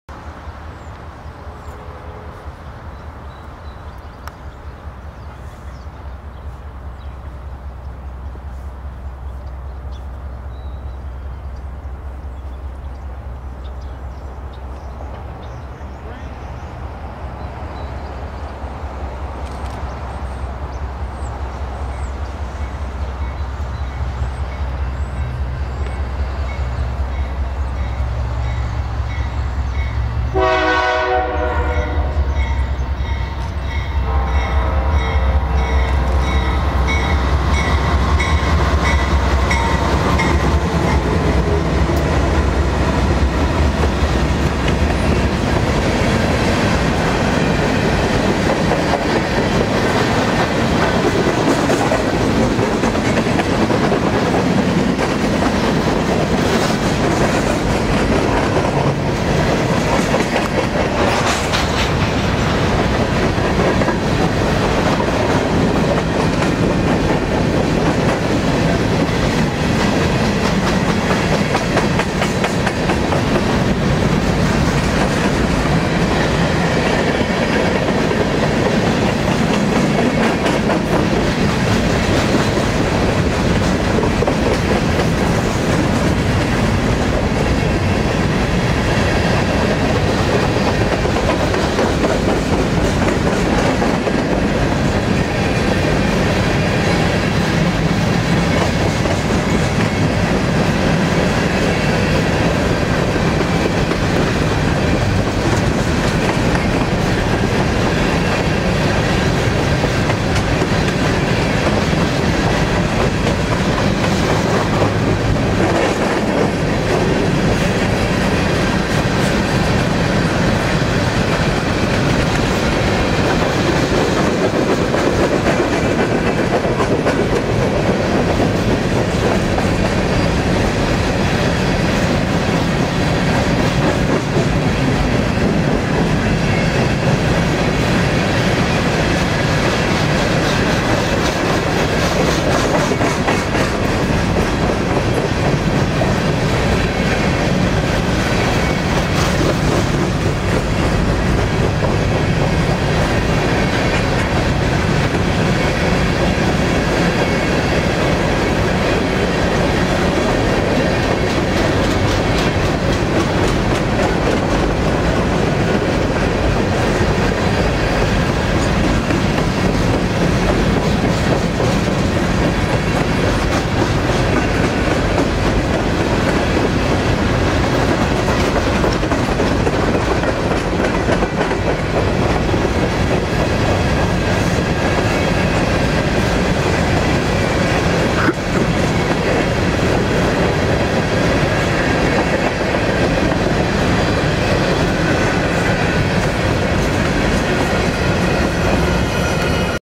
BNSF 4535 @ Old Town Tacoma, WA w Canon HF11
BNSF 4535, 4162, and 5339 lead a northbound empty grain train through Old Town Tacoma, WA on BNSF's Seattle Sub. A 23 March 2012 video.